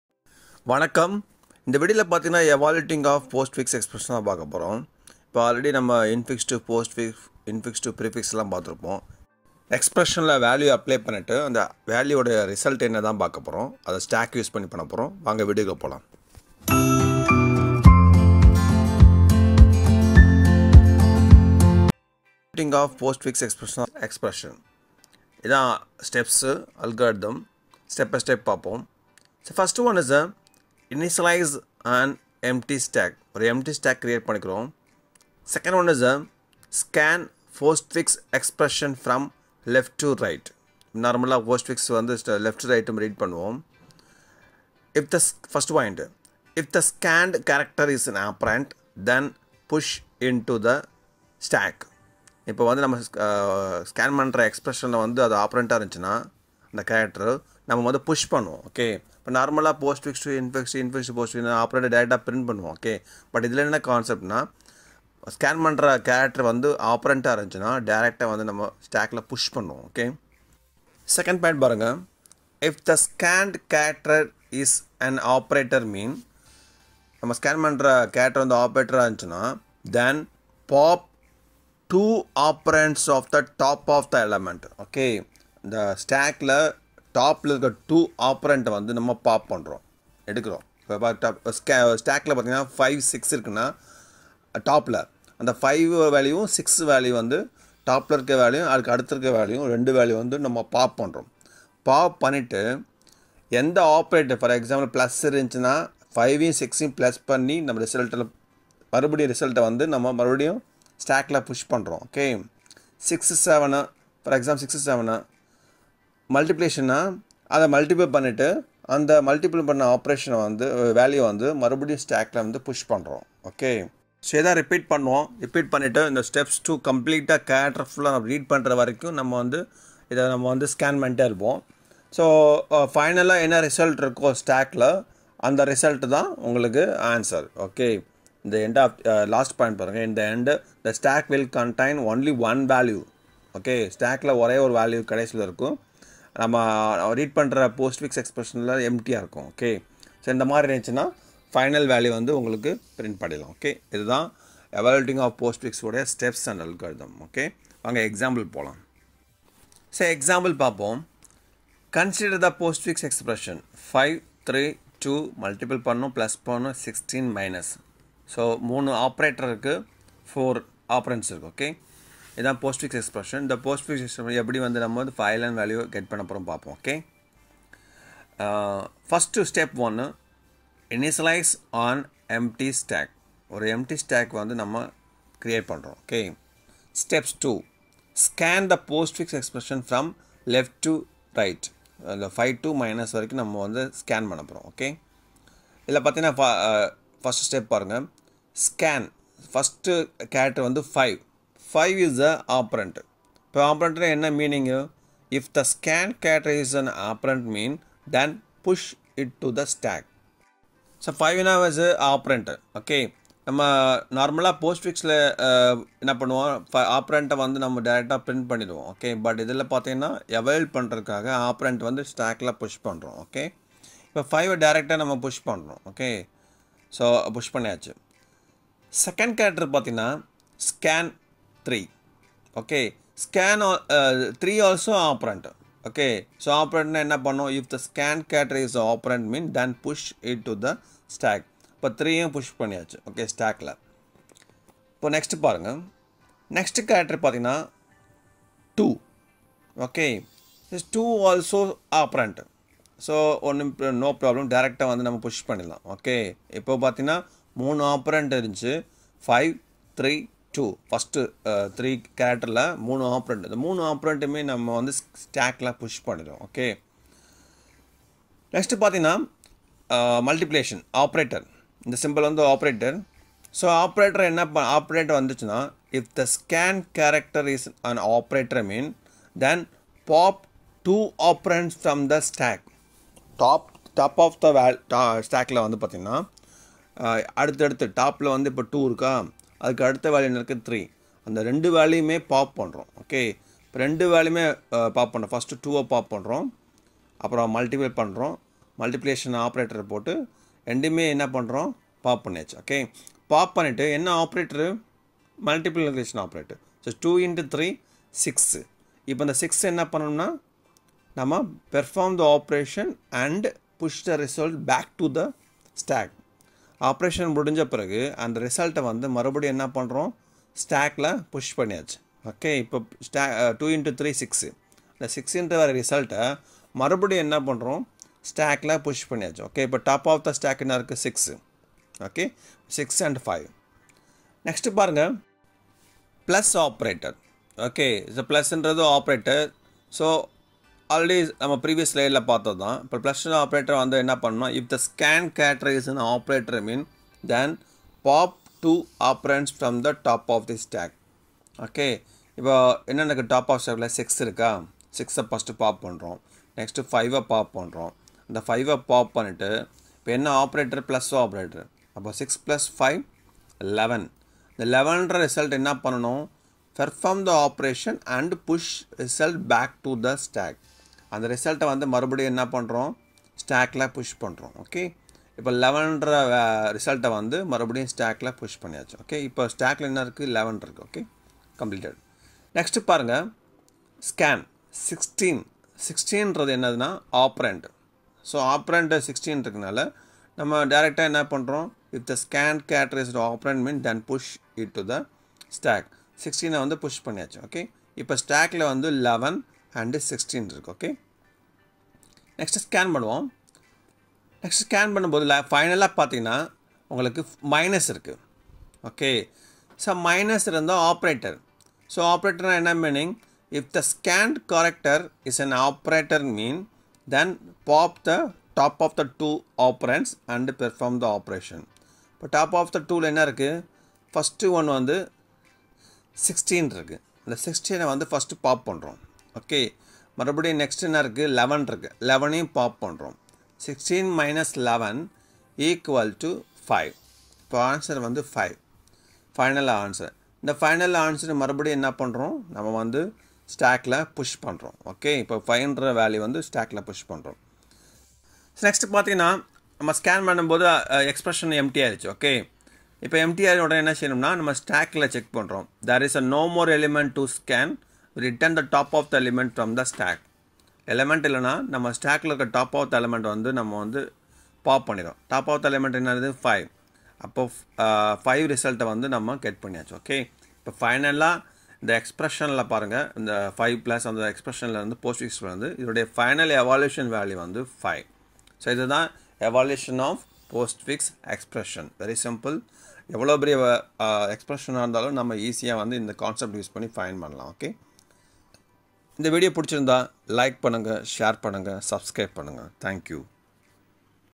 Evaluation of Postfix Expression in Data Structure (Tamil)
Evaluation of Postfix  in Data Structure in Tamil
-Dr.R.Satheeskumar.
5 3 2 * + 16 -
=5